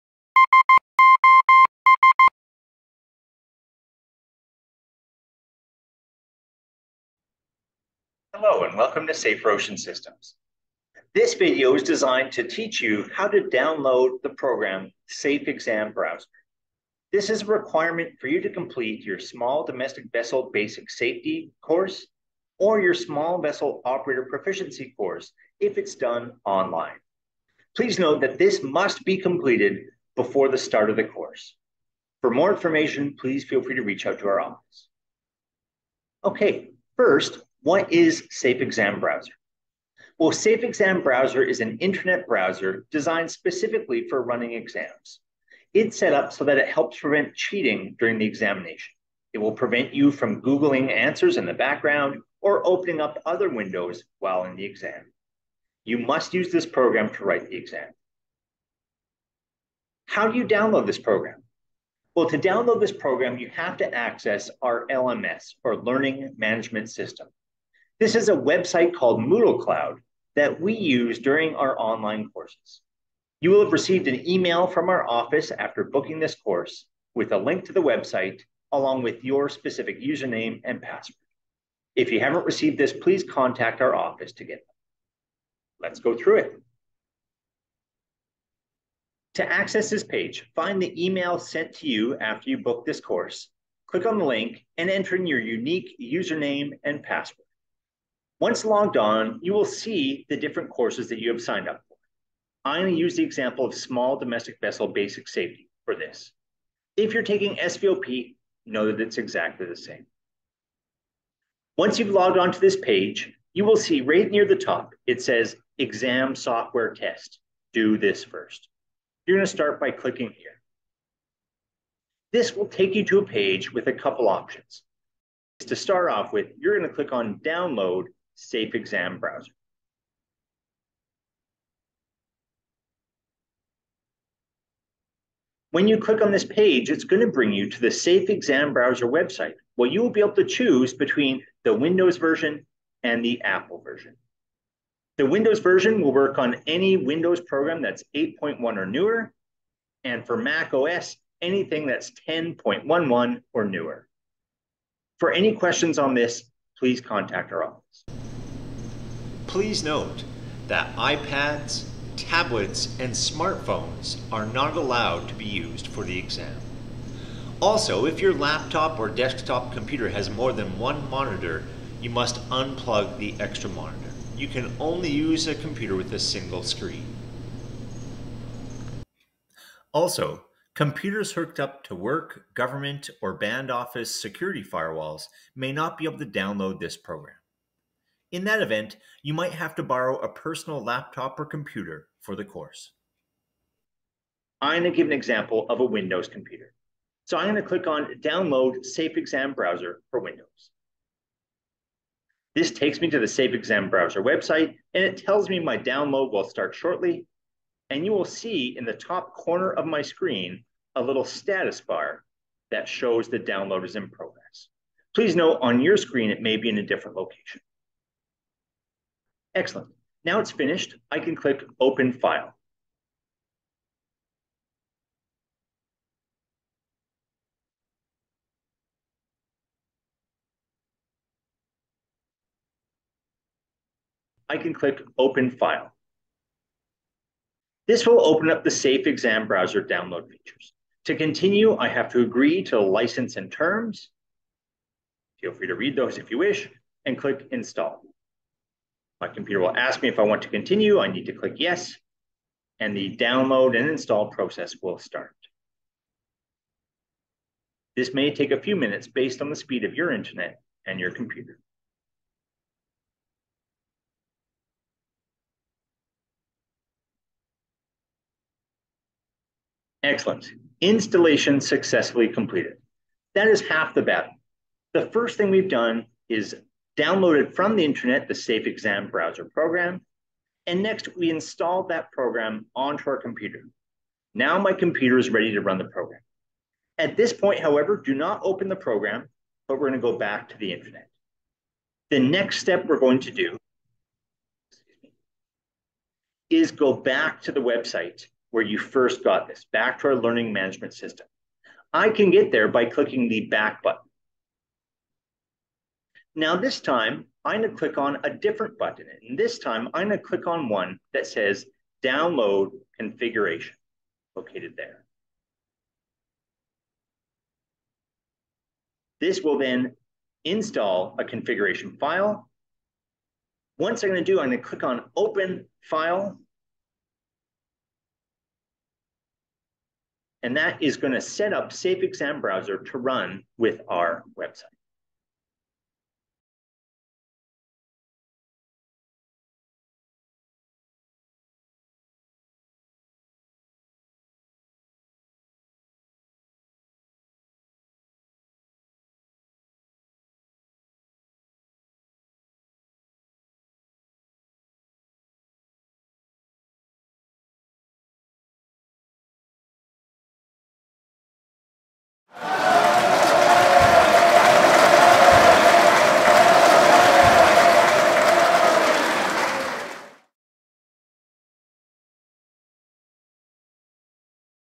Downloading Safe Exam Browser
Instructions for Downloading Safe Exam Browser onto your computer for the online SVOP or SDVBS exam with Safer Ocean Systems.